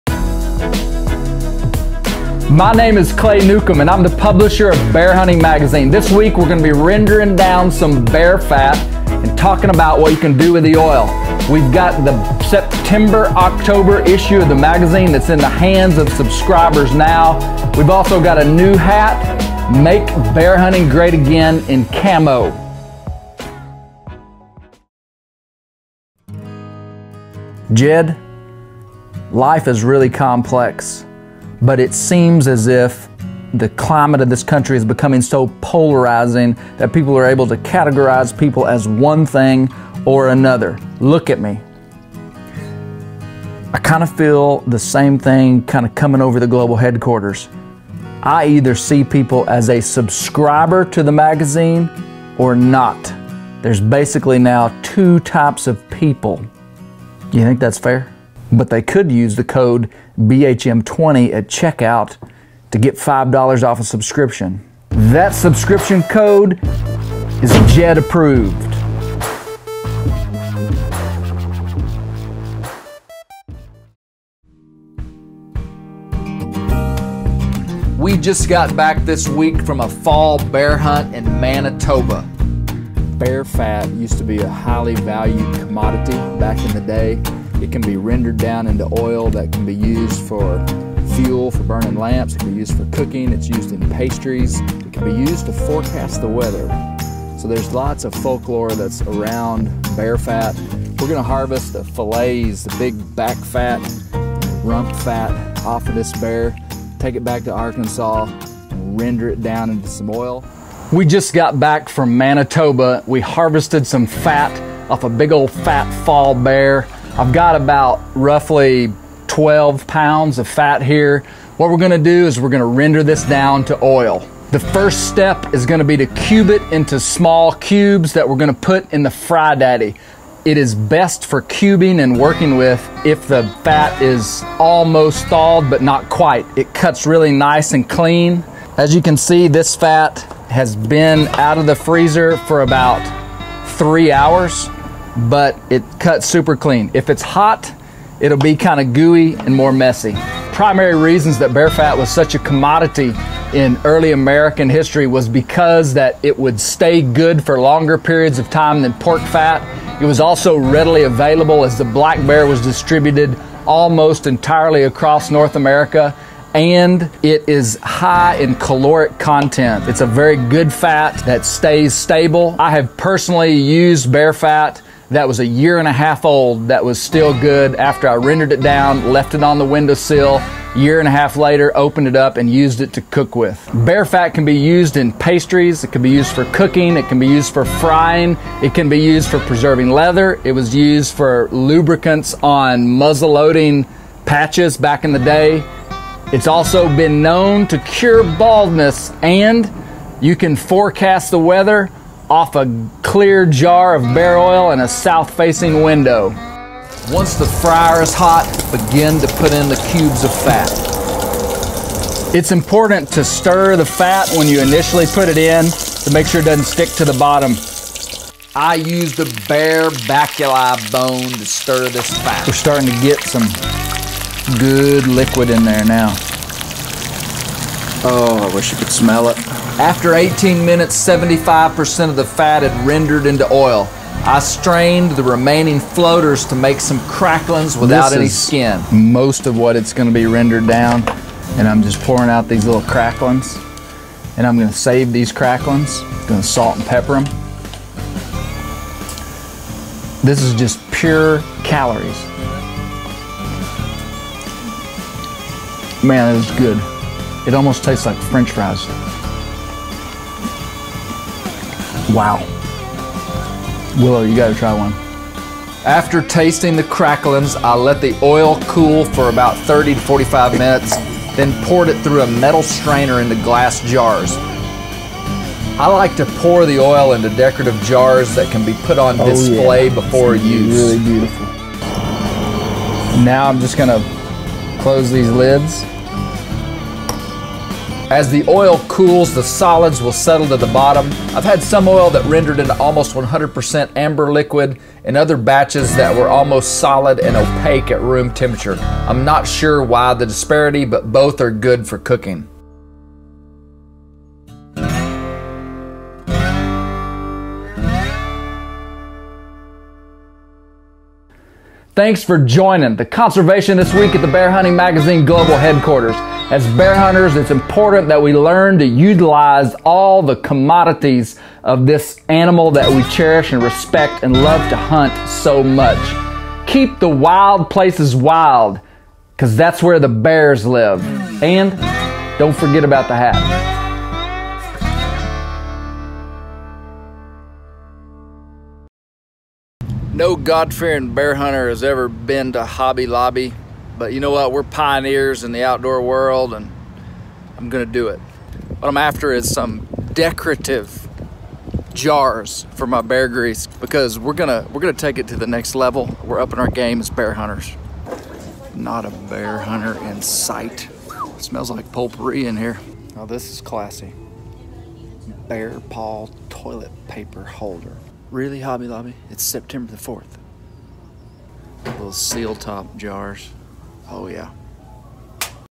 Vlog #27: How to Render Bear Fat
This week Clay shows how to harvest fat off a bear in the field and then render the bear fat into oil.